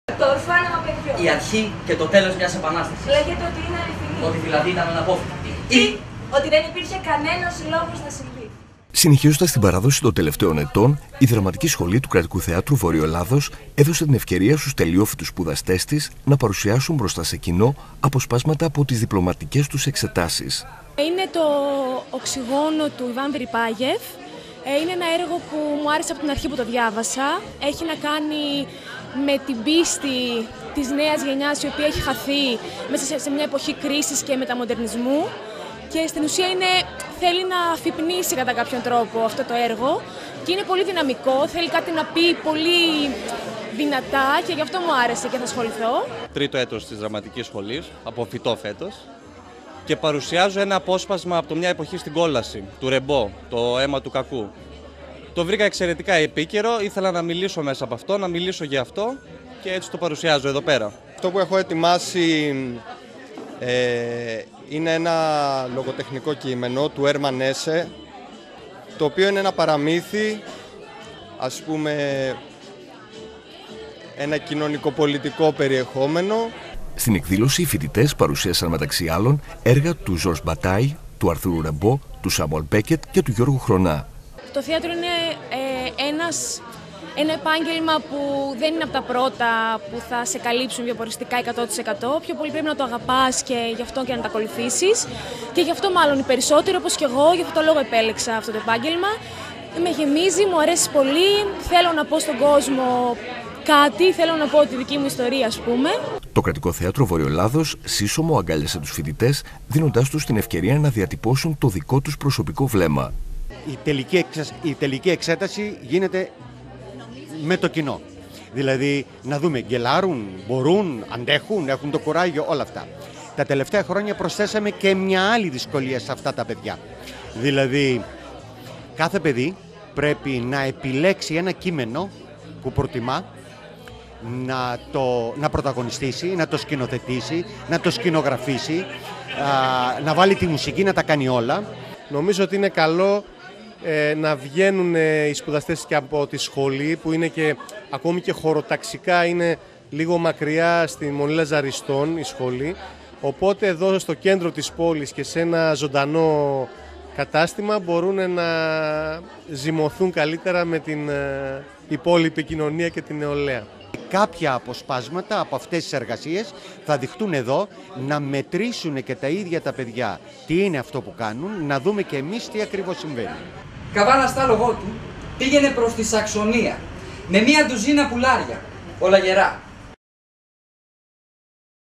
ΤΕΛΕΙΟΦΟΙΤΟΙ ΤΗΣ ΔΡΑΜΑΤΙΚΗΣ ΣΧΟΛΗΣ ΚΘΒΕ ΣΤΗ "ΖΩΓΙΑ"(TV100-030517)
Μια ξεχωριστή παράσταση της Δραματικής Σχολής του Κρατικού Θεάτρου Βορείου Ελλάδος παρουσίασε αποσπάσματα από τις ατομικές διπλωματικές εξετάσεις των τελειοφοίτων της. Η εκδήλωση με τίτλο “35 Μέρες Πριν..” εντυπωσίασε τους θεατές για την αρτιότητα και τον επαγγελματισμό των νέων εκκολαπτόμενων ηθοποιών.
Ρεπορτάζ: Διογένης Μαριόλας
Κάμερα: Σάκης Σώνης
Μοντάζ: Πάνος Τσακίρης
Μιλούν: ΚΑΤΕΡΙΝΑ ΠΛΕΞΙΔΑ(Τελειόφοιτη Δραμ. Σχολής ΚΘΒΕ),
ΝΙΚΟΣ ΜΗΛΙΑΣ(Τελειόφοιτος  Δραμ. Σχολής ΚΘΒΕ),
ΓΙΑΝΝΗΣ ΚΑΡΑΜΠΑΜΠΑΣ(Τελειόφοιτος  Δραμ. Σχολής ΚΘΒΕ),
ΓΙΑΝΝΗΣ ΡΗΓΑΣ(Καθηγητής Δραμ.Σχολής ΚΘΒΕ),
ΑΡΗΣ ΣΤΥΛΙΑΝΟΥ(Πρόεδρος Δ.Σ. Κ.Θ.Β.Ε.)